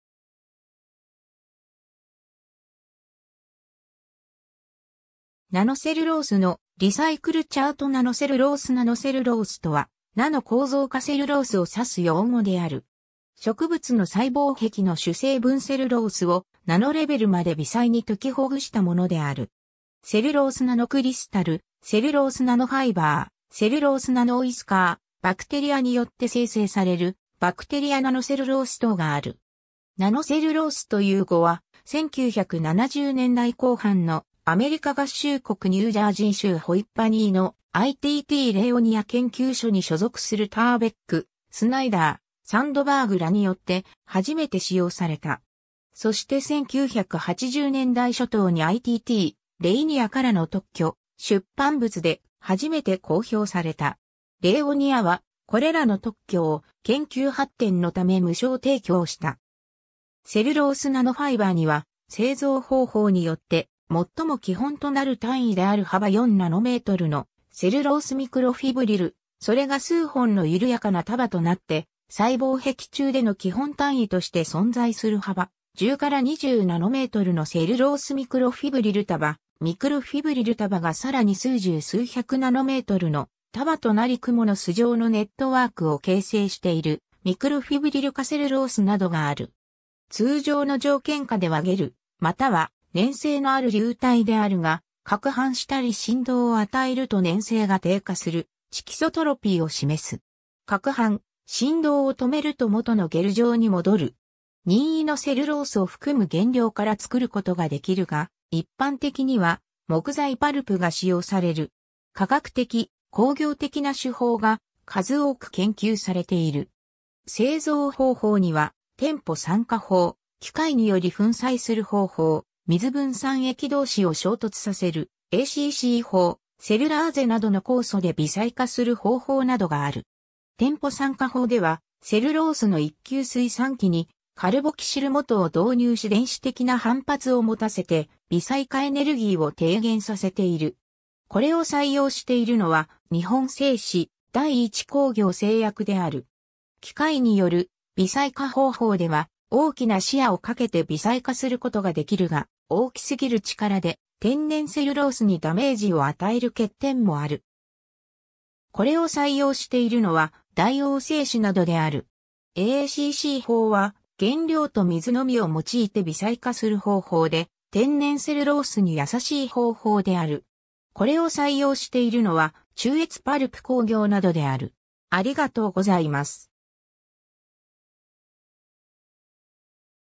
ナノセルロース
ナノセルロースのリサイクルチャート ナノセルロース ナノセルロース（英語：Nanocellulose）とは、ナノ構造化セルロースを指す用語である。
植物の細胞壁の主成分セルロースをナノレベルまで微細に解きほぐしたものである。
セルロースナノクリスタル（CNCまたはNCC）、セルロースナノファイバー（CNF）、セルロースナノウィスカー（CNW）、バクテリアによって生成されるバクテリアナノセルロース等がある。
 ナノセルロースという語は、1970年代後半のアメリカ合衆国ニュージャージー州ホイッパニーの ITT レイオニア（Rayonier）研究所に所属するTurbak、Snyder、Sandbergらによって初めて使用された。
そして1980年代初頭にITT Rayonierからの特許・出版物で初めて公表された。
レイオニアは、これらの特許を研究発展のため無償提供した。
 セルロースナノファイバーには製造方法によって、もっとも基本となる単位である幅 4 nmのセルロースミクロフィブリル（シングルセルロースナノファイバー）、それが数本のゆるやかな束となって細胞壁中での基本単位として存在する幅10～20 nmのセルロースミクロフィブリル束、ミクロフィブリル束がさらに数十～数百 nmの束となりクモの巣状のネットワークを形成しているミクロフィブリル化セルロース（MFC）などがある。
 通常の条件下ではゲルまたは粘性のある流体であるが、攪拌したり振動を与えると粘性が低下するチキソトロピーを示す。
攪拌・振動を止めると元のゲル状に戻る。
 任意のセルロースを含む原料から作ることが出来るが、一般的には木材パルプが使用される。
化学的・工業的な手法が数多く研究されている。
 製造方法にはTEMPO酸化法、機械により粉砕する方法、水分散液同士を衝突させるACC法、セルラーゼなどの酵素で微細化する方法などがある。
TEMPO酸化法ではセルロースの一級水酸基にカルボキシル基を導入し電子的な反発を持たせて微細化エネルギーを低減させている。
これを採用しているのは日本製紙、第一工業製薬である。
機械による微細化方法では大きなシアを掛けて微細化することができるが大きすぎる力で天然セルロースにダメージを与える欠点もある。
これを採用しているのは大王製紙などである。
ACC法は原料と水のみを用いて微細化する方法で天然セルロースに優しい方法である。
これを採用しているのは中越パルプ工業などである。